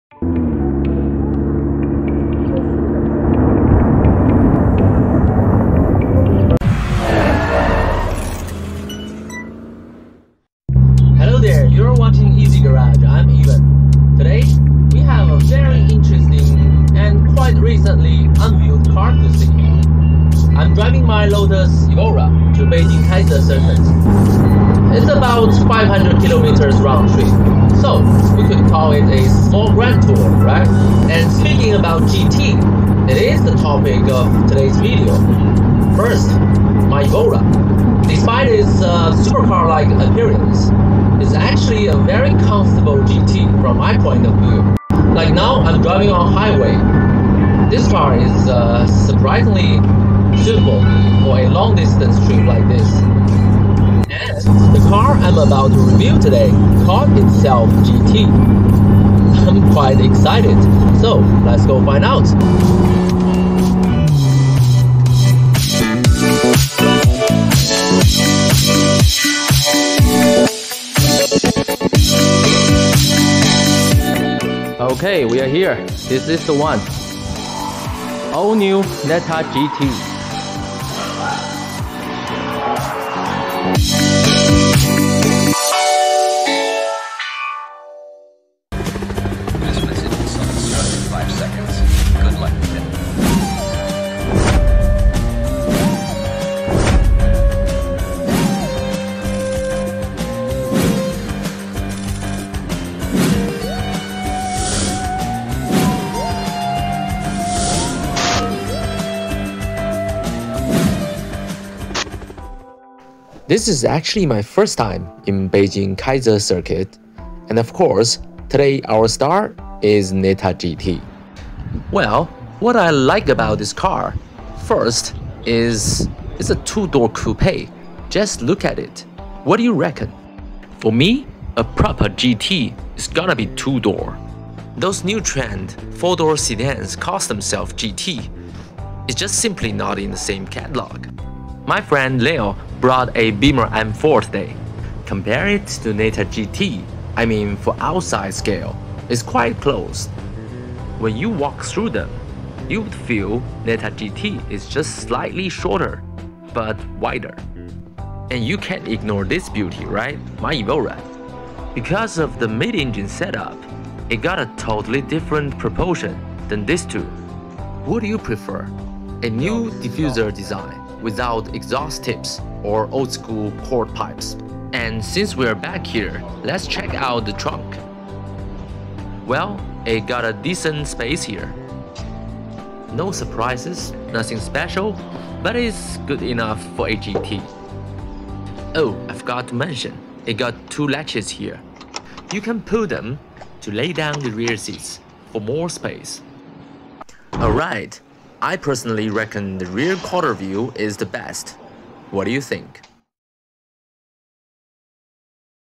a Chinese new electric coupe called GT
today Im driving my evora. a none traditional GT, to Beijing to test drive another EV car called GT. let's see what gonna happen